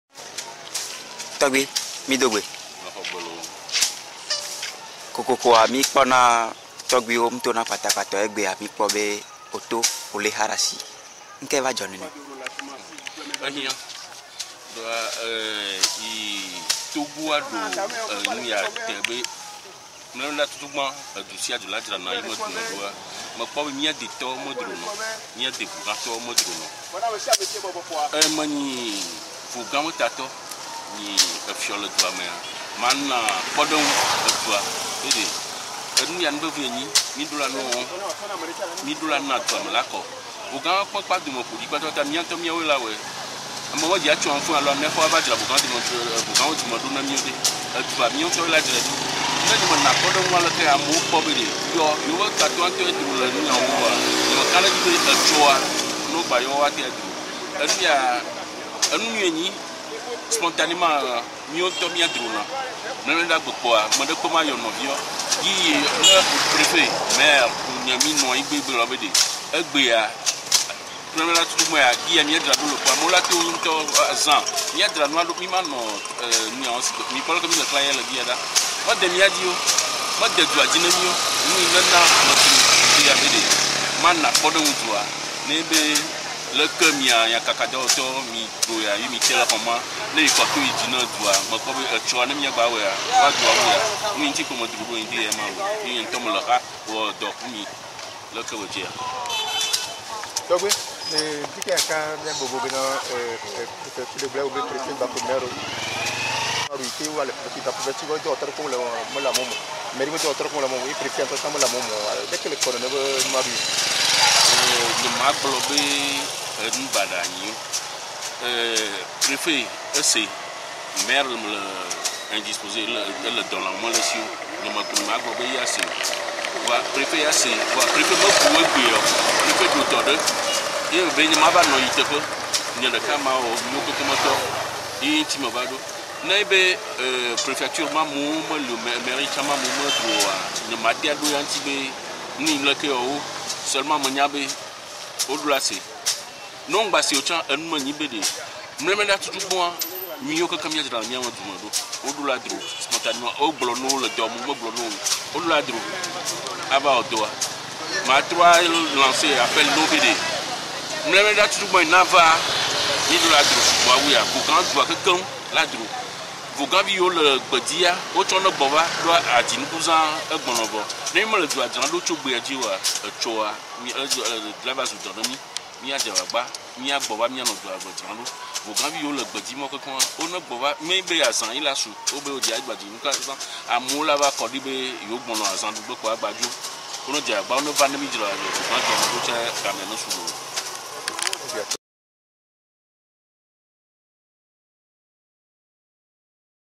Echos de Bè: Togbui Odima KALIPE III "lance un appel pour l'opération Ville Propre, devoir civique"
Togo: Ce 01/07/2017, Le MMLK s'inscrit dans cette dynamique et appréci cette initiative qui avait déjà fait ces preuves dans notre pays. Le MMLK déplorel'absence des maitres de lieux et ces colaborateurs, ni les agents de la Mairi, ni les fonctionnaires publics, ni les habitants de la ville, ne se sont pas déplacés pour opération.